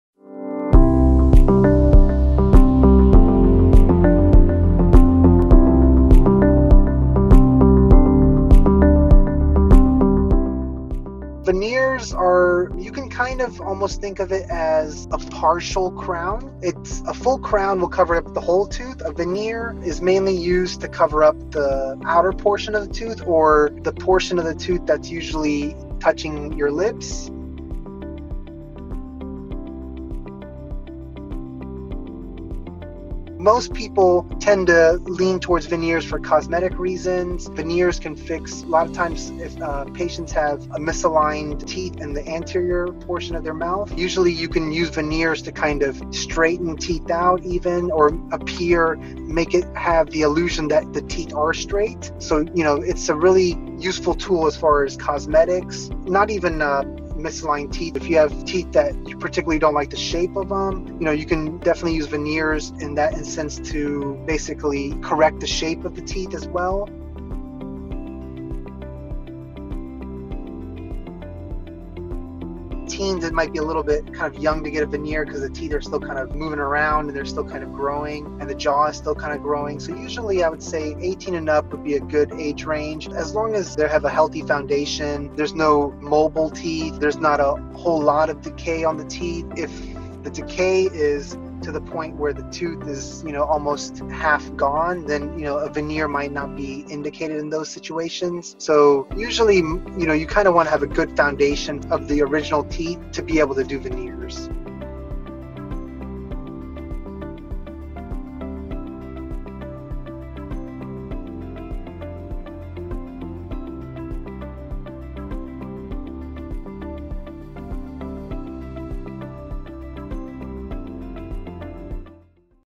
Dental veneers at Vivid Dental explained by Dr. Monti Harpalani
Are you looking to get that perfect Hollywood smile? Then dental veneers are what you should get. Watch this video to learn more about dental veneers, as explained by Dr. Monti Harpalani from Vivid Dental.